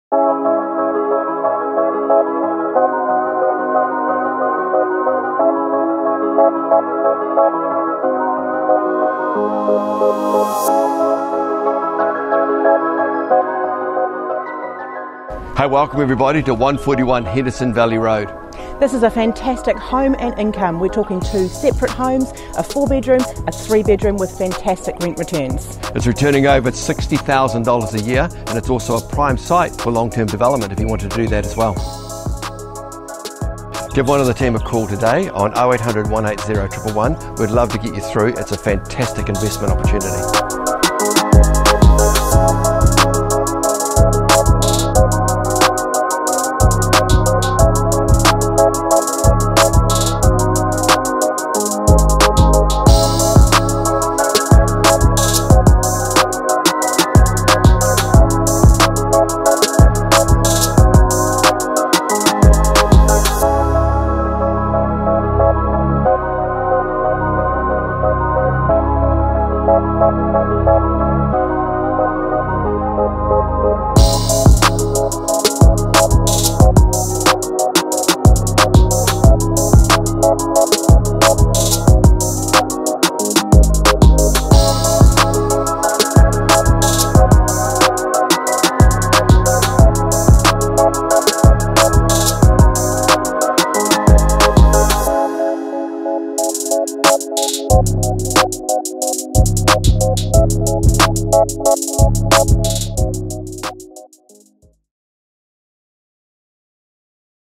141 Henderson Valley Road - Home and Income
A fantastic investment opportunity - a 4 bedroom weatherboard home and a 3 bedroom home at the rear of the site.  Contact Team Malloy on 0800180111 to receive more information.